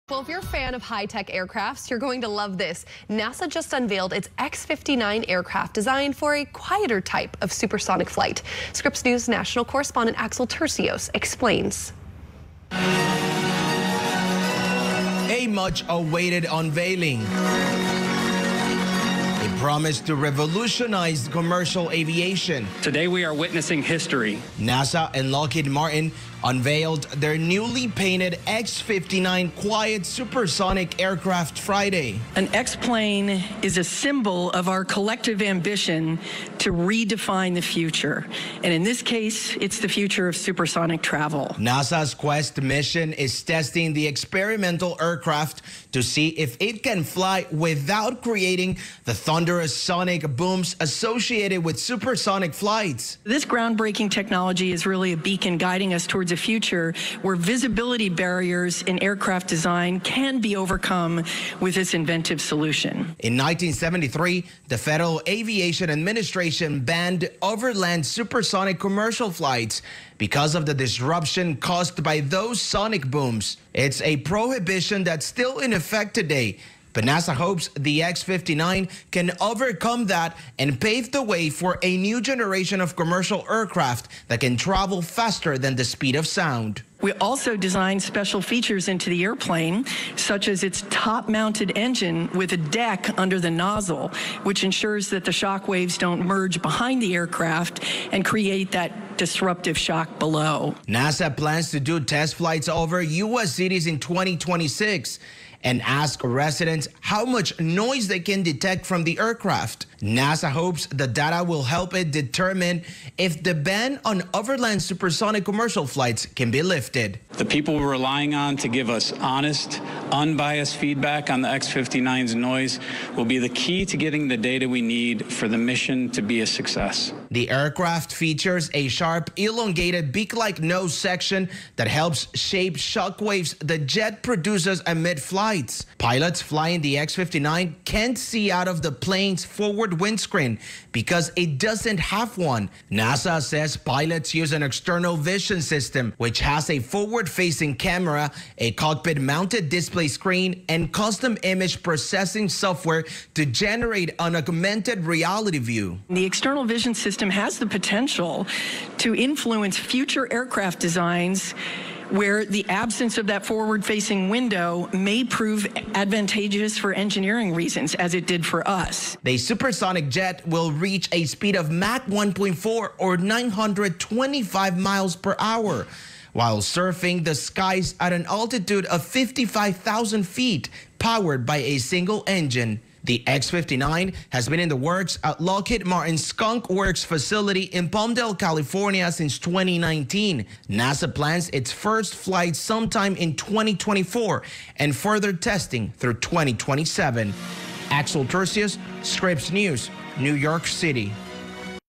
NASA unveils its Lockheed Martin-made X-59 quiet supersonic jet
NASA plans to do test flights over U.S. cities in 2026 and ask residents how much noise they can detect from the aircraft.